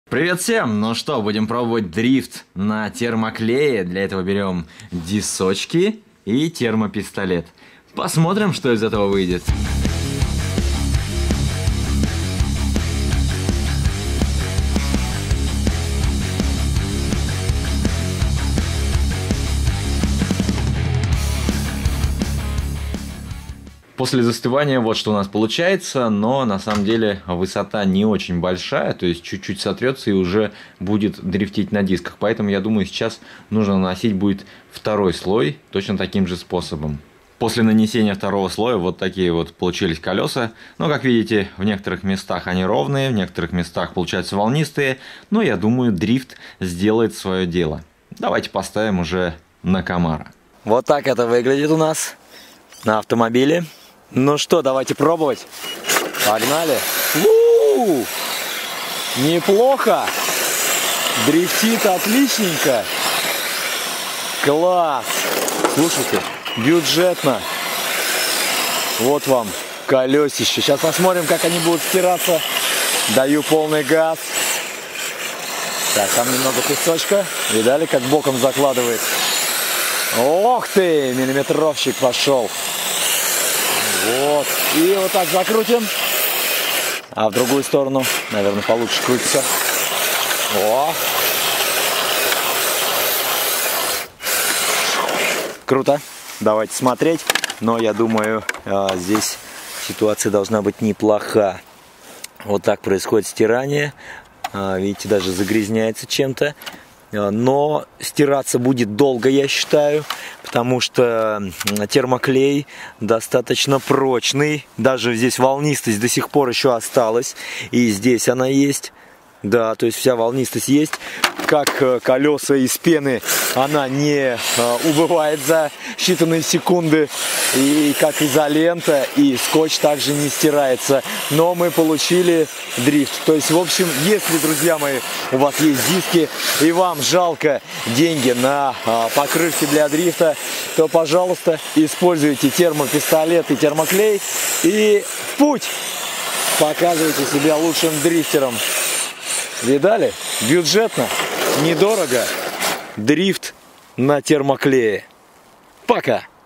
ТЕРМОКЛЕЙ вместо шин ... Пробую дрифт! RC drift
Дрифт на термоклее? Проверим, реально ли это! Проверяем на Chevrolet Camaro. Устроим RC Drift